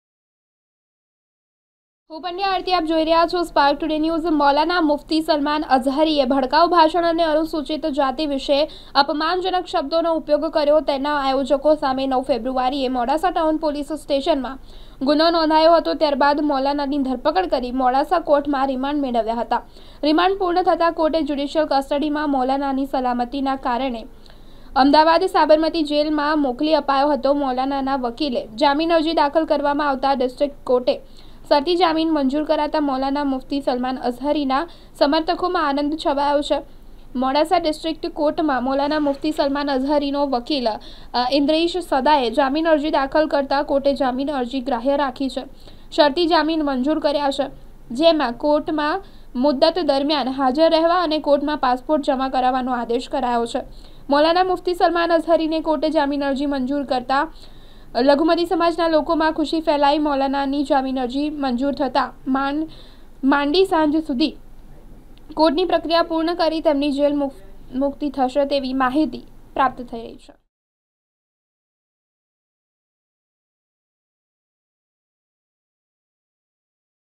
મૌલાના મુફ્તી સલમાન અઝહરીની કોર્ટે જામીન અરજી મંજૂર કરી 2024 | Spark Today News Vadodara
મૌલાના મુફ્તી સલમાન અઝહરીની કોર્ટે જામીન અરજી મંજૂર કરી